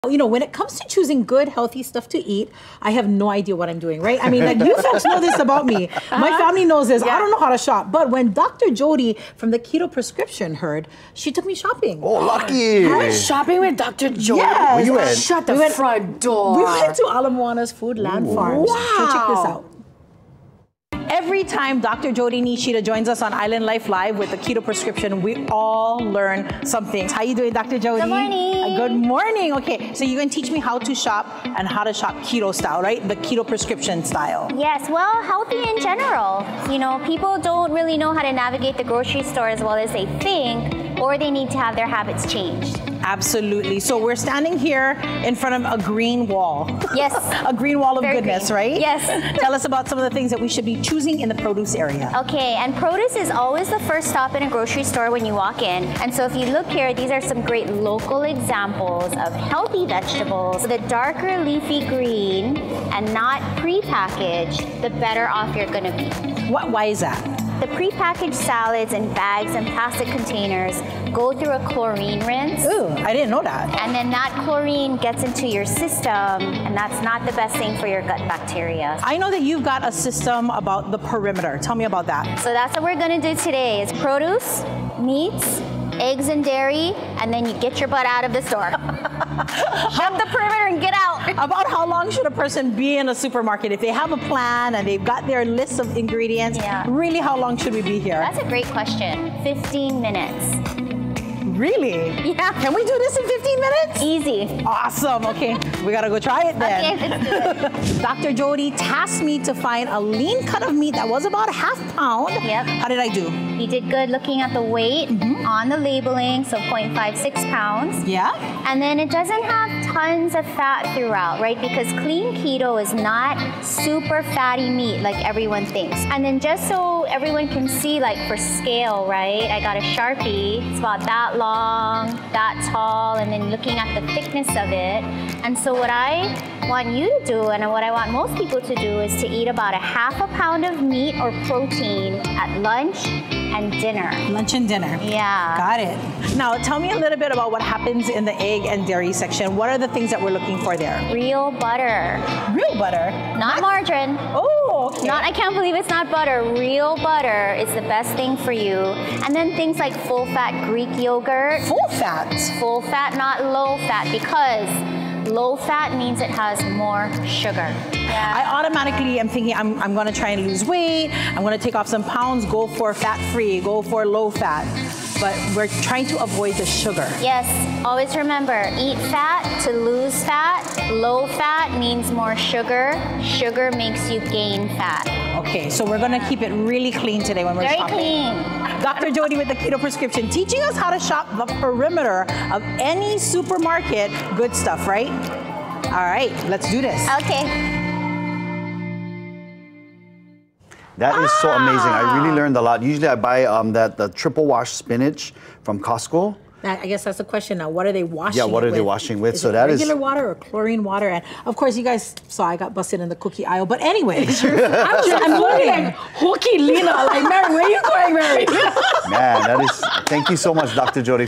The Keto Prescription: Grocery Shopping
Dr. Jodi Nishida with The Keto Prescription shares her tips for healthy grocery shopping.